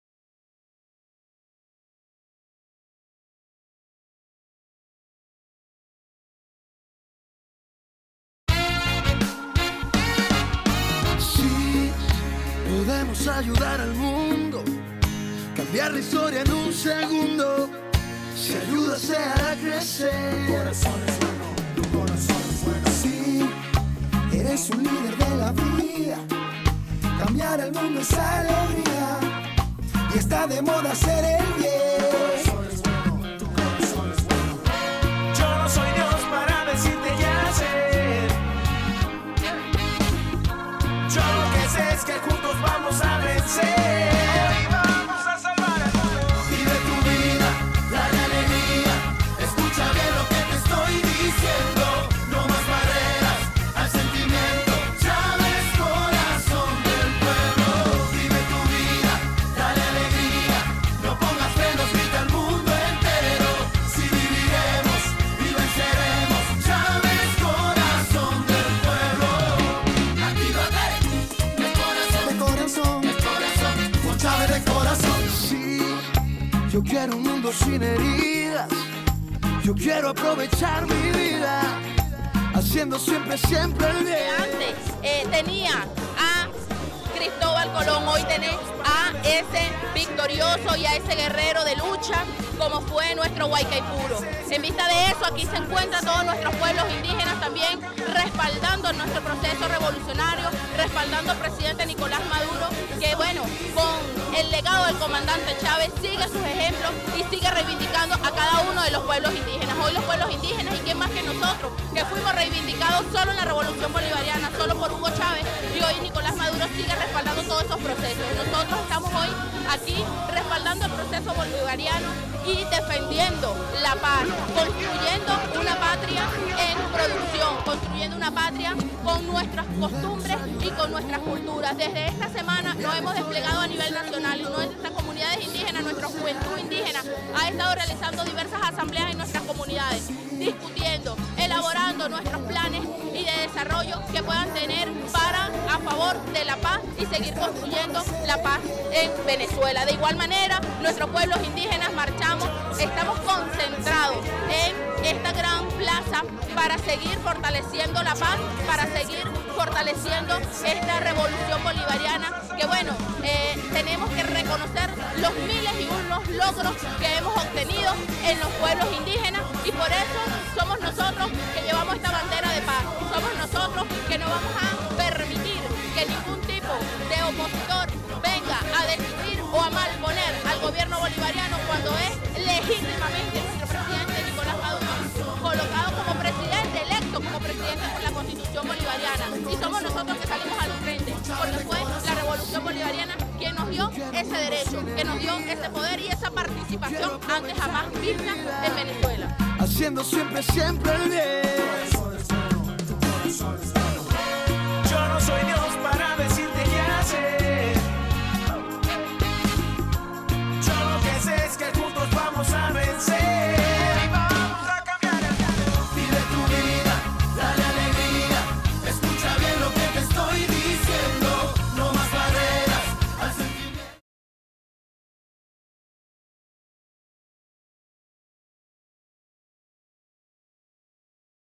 Ministra Clara Vidal
Ministra del Poder Popular Para Los Pueblos Indígenas Vidal Clara
En Plaza De La Resistencia Indígena
Pueblos Indígenas Marcha en apoyo al Pueblo de Bolivia y por la Paz de Venezuela.